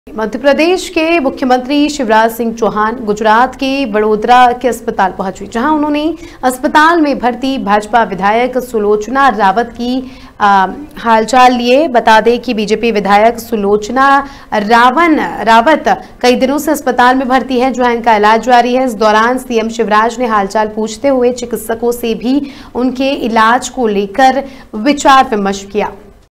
मध्यप्रदेश के मुख्यमंत्री शिवराज सिंह चौहान गुजरात के, वड़ौदरा के अस्पताल पंहुचे
वड़ोदरा - मध्यप्रदेश के मुख्यमंत्री शिवराज सिंह चौहान गुजरात के, वड़ौदरा के अस्पताल पंहुचे